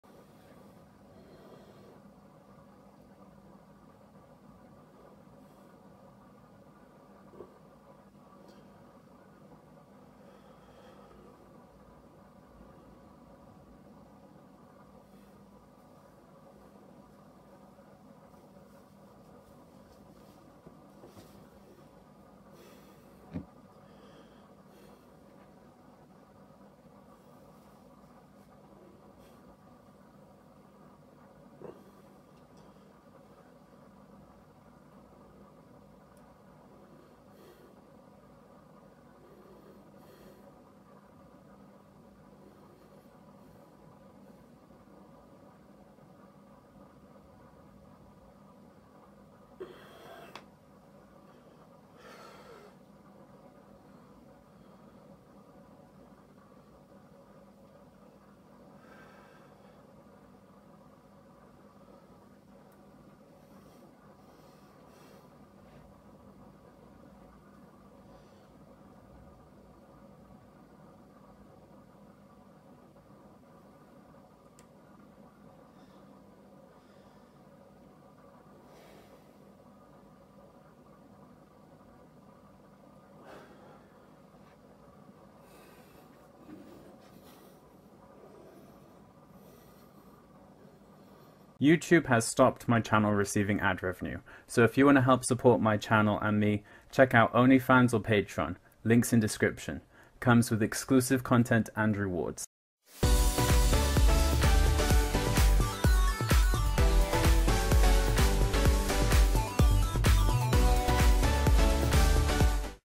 250lbs offseason bodybuilder morning flexing
"BIGGEST Bodybuilder onlyfans sale! -- 3 year anniversary"
woke up and decided to do some morning posing, feeling pretty good, arms look a lot thicker, especially from one of my Instagram posts last night! no way you guys are seeing any lower, maybe when i cut back down, no